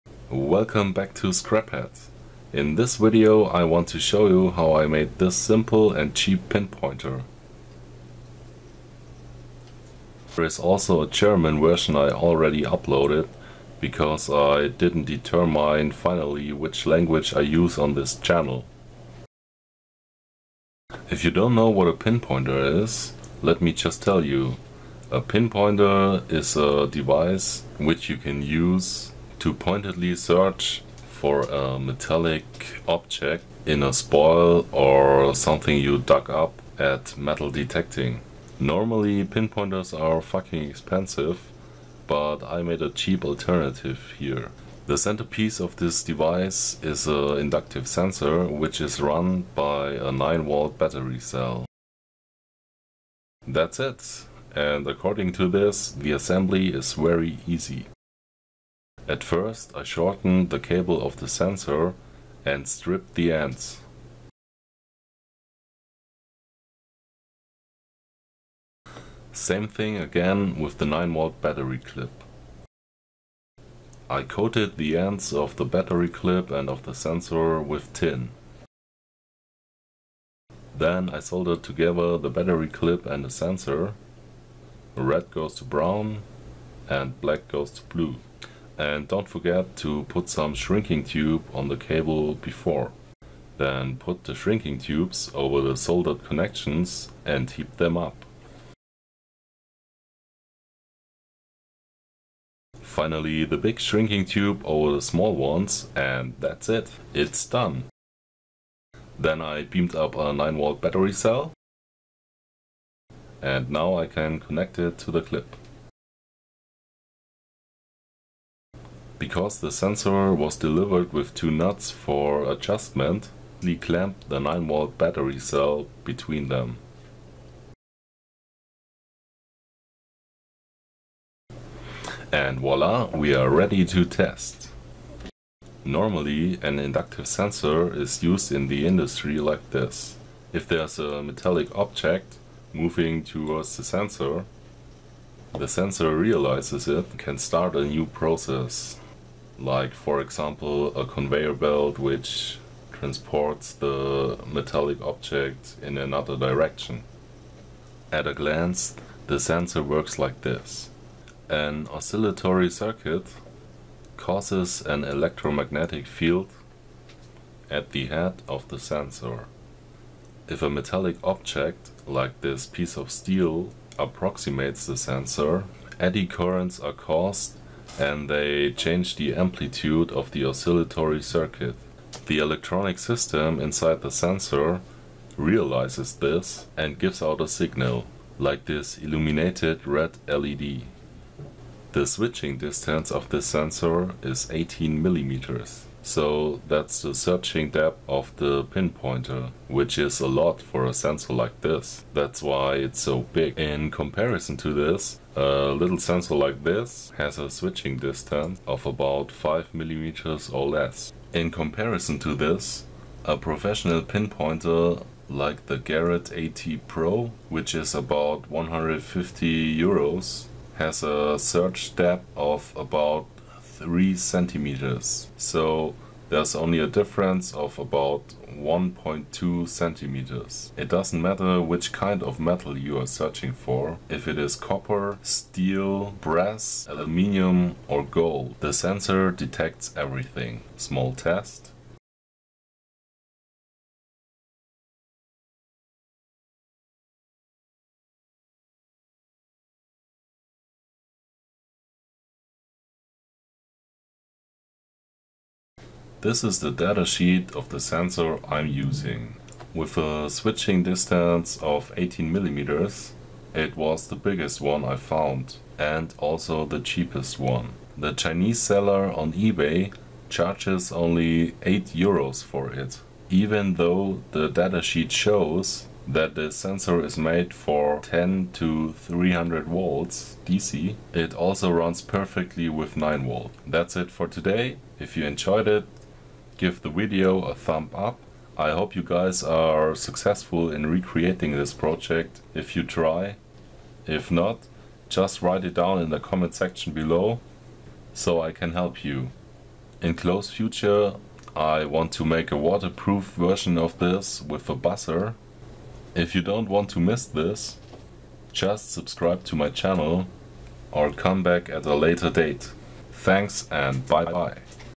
Homemade simple and cheap PINPOINTER
In this video I show you, how I made a simple pinpointer for metal detecting. It has a searching depth of 18 mm and material costs about 10€. Perfectly for beginners who don´t want to pay 150€ for a device like this.

6-36 V DC / Distance 8mm price ca. 8€
10-30 V DC / Distance 15mm price ca. 25€

If you use a 3 wire sensor, just put brown to plus and blue to minus:

6-36V DC / Distance 18mm price ca. 14€
6-36V DC / Distance 15mm price ca. 11€